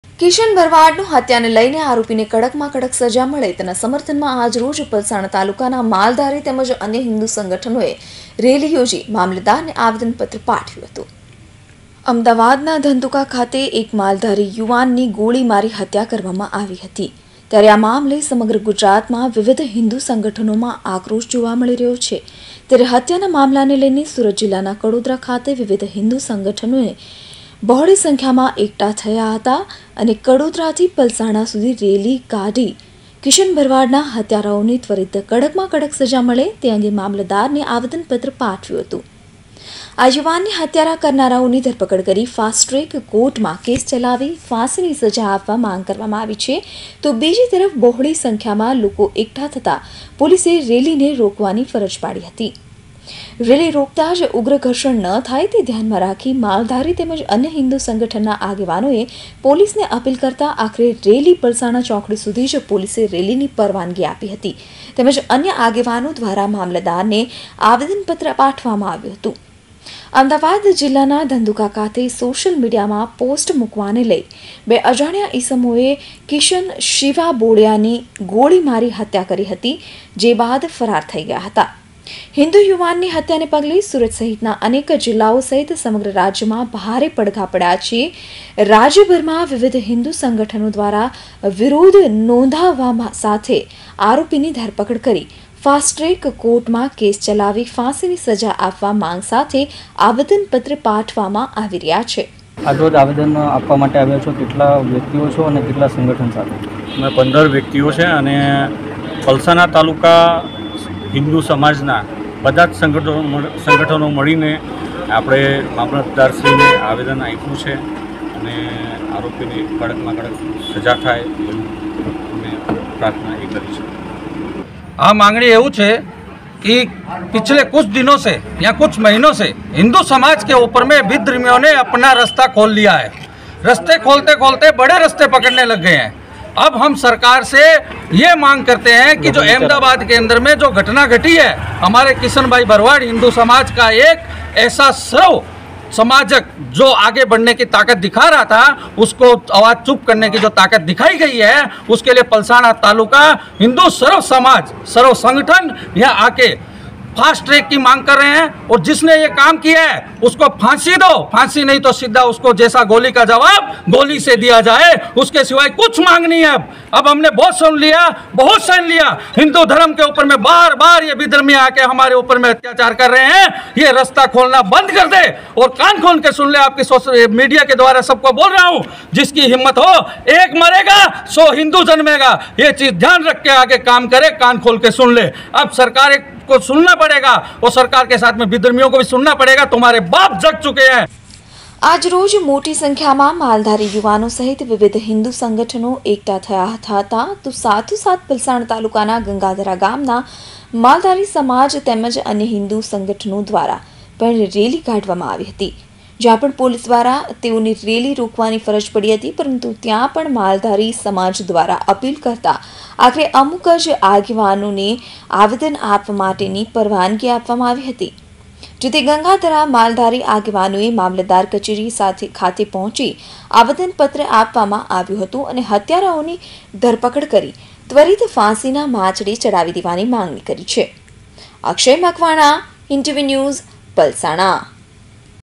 કિશન ભરવાડા હત્યા મામલે પલસાણા તાલુકમાં નીકળી રેલ
માલધારી સમાજ દ્વારા રેલી કાઢી મામલતદારને આવેદનપત્ર પાઠવ્યું
આરોપીઓને સખ્તમાં સખ્ત સજા આપવા માંગ કરાઈ